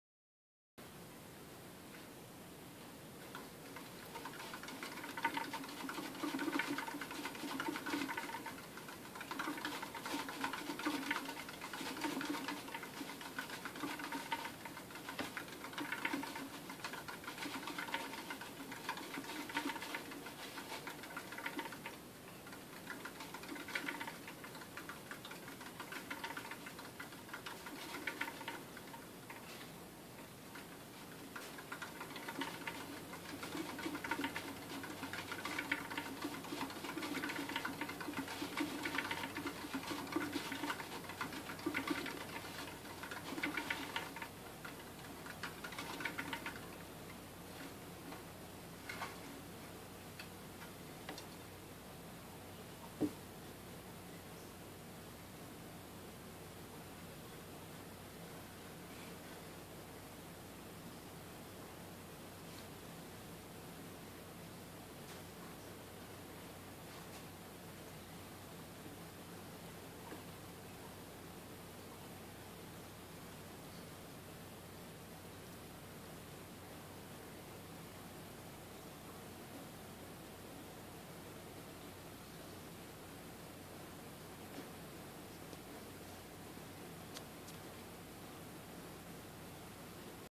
Pygmy Hedgehog Male with Wheel cute funny animal video youtube
Hedgehogs need exercise, you can let them run around the room or give them a large wheel.